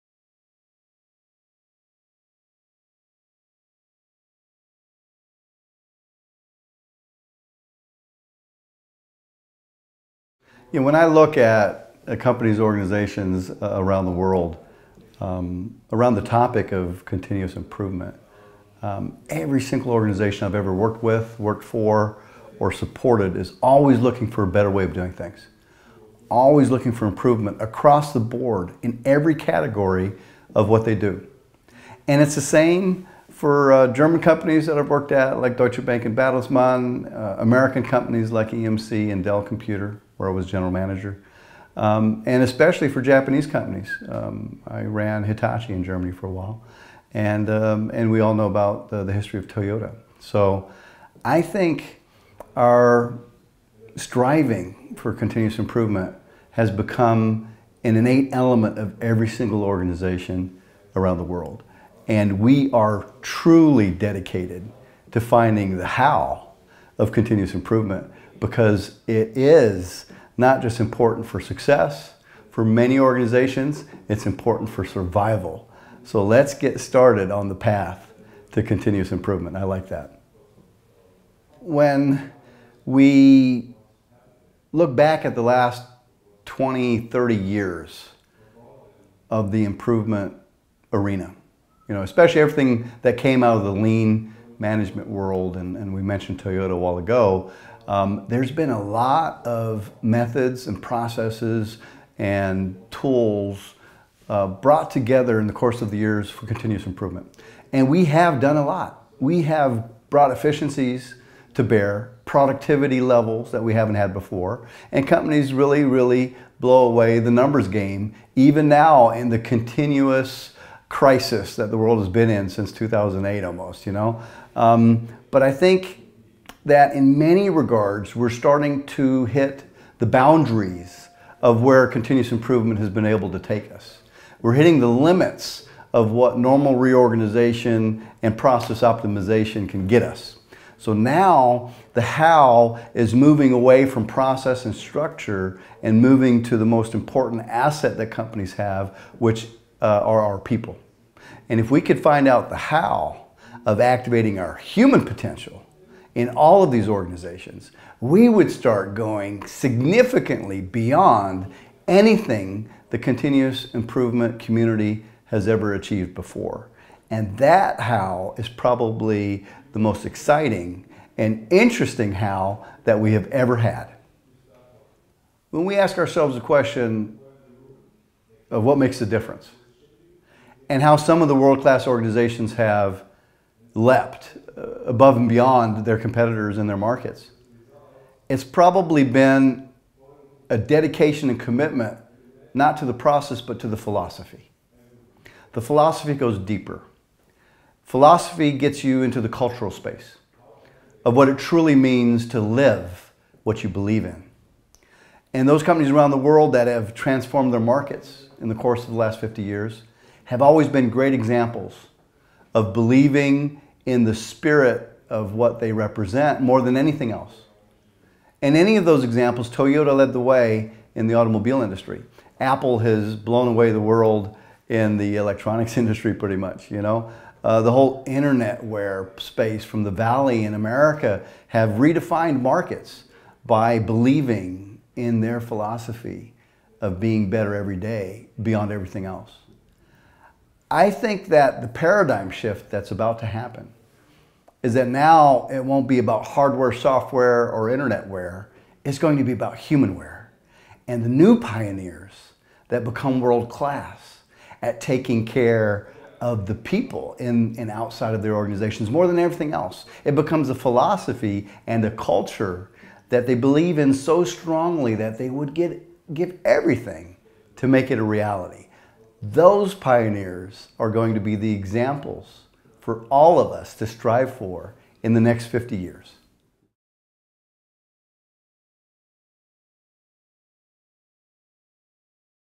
Patrick Cowden: Rekommendationer till svenska företag som vill förbättra sin verksamhet
Patrick Cowden, founder of Beyond Leadership, gives advice to managers who want to succeed with continuous improvement.